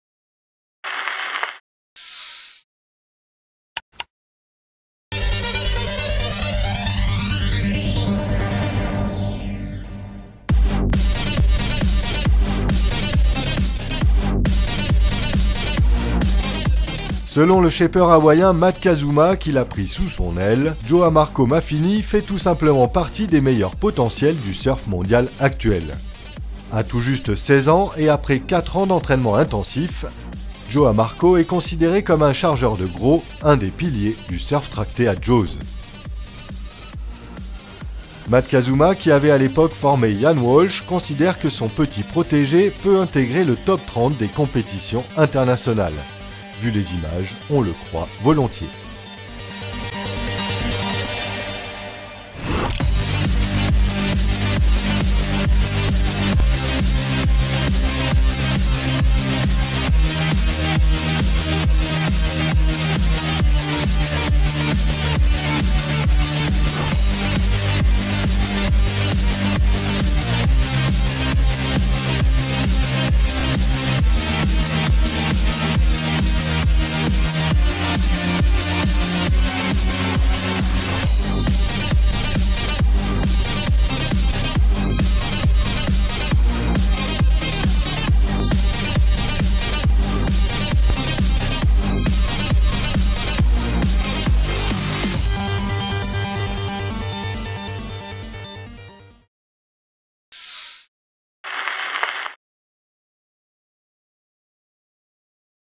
Surfeur de gros : Joao Marco Maffini le protégé de Matt Kazuma
Selon le shapeur hawaïen Matt Kazuma qui l'a pris sous son aile, Joao Marco Maffini fait tout simplement partie des meilleurs potentiels du surf mondial actuel!